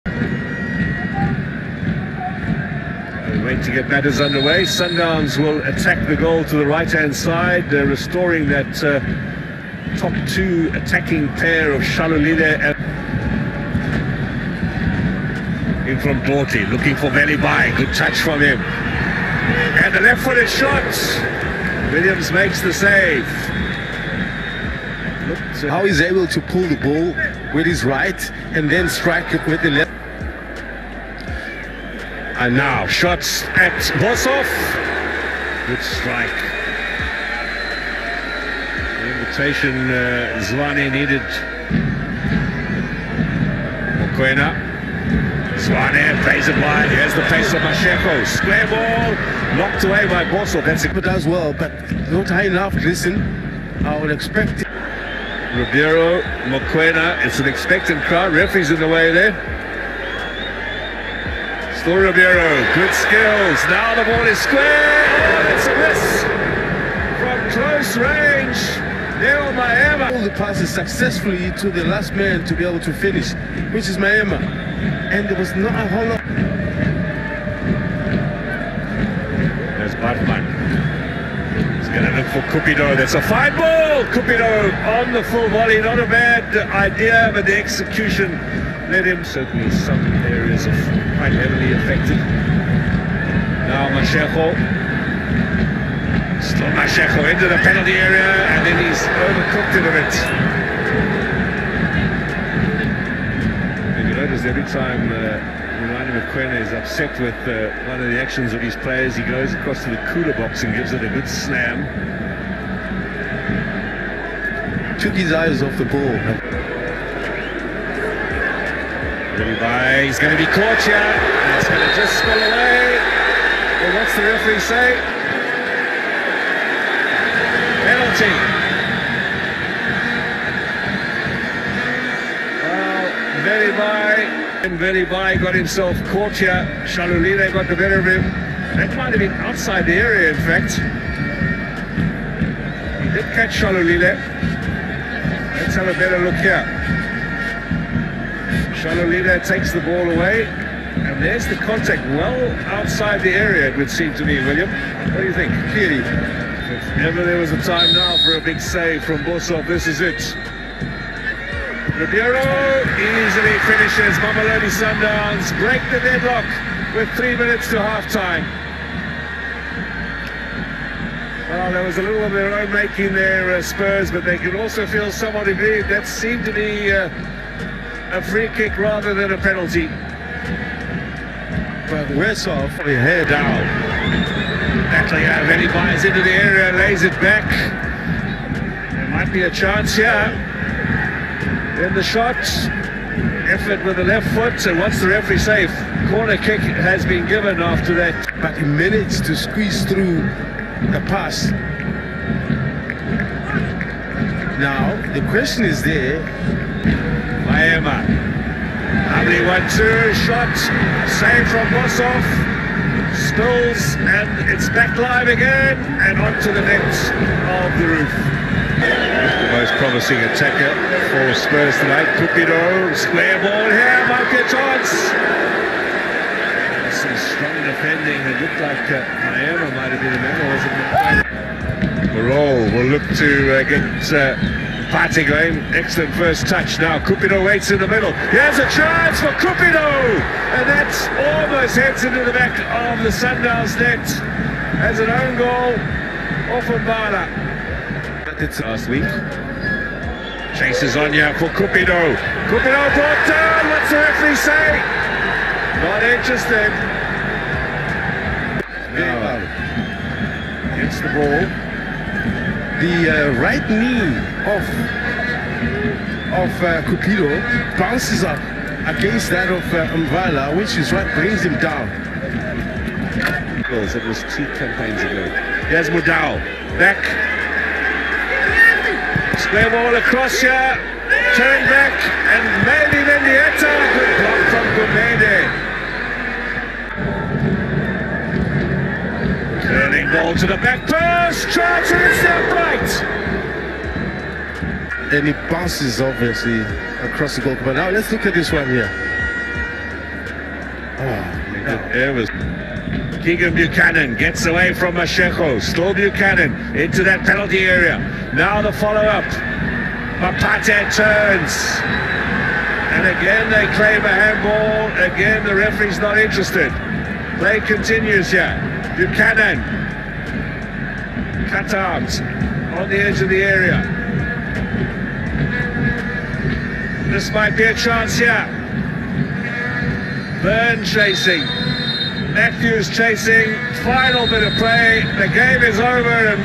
Cape Town Spurs Vs Mamelodi Sundowns Dstv Premiership
Watch key moments from Mamelodi Sundowns's 1 - 0 win with Cape Town Spurs  in the Dstv Premiership, with goal from Lucase Ribeiro Costa to secure 3 point at Athlone Staduim.

Chris kupanga